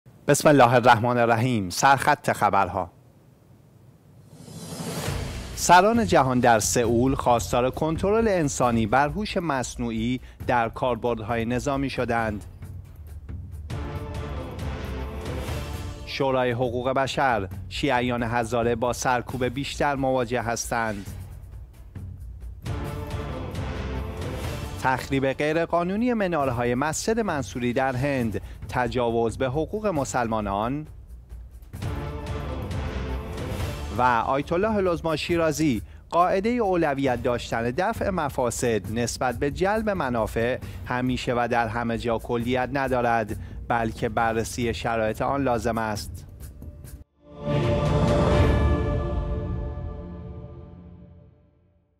سر خط خبر ها
-  سران جهان در سئول خواستار کنترل انسانی بر هوش مصنوعی در کاربردهای نظامی شدند

- شورای حقوق بشر: شیعیان هزاره با سرکوب بیشتر مواجه هستند

- تخریب غیرقانونی مناره‌های مسجد منصوری در هند؛ تجاوز به حقوق مسلمانان

 - آیت الله العظمی شیرازی: قاعده اولویت داشتن دفع مفاسد نسبت به جلب منافع، همیشه و در همه جا کلیت ندارد؛ بلکه بررسی شرایط آن لازم است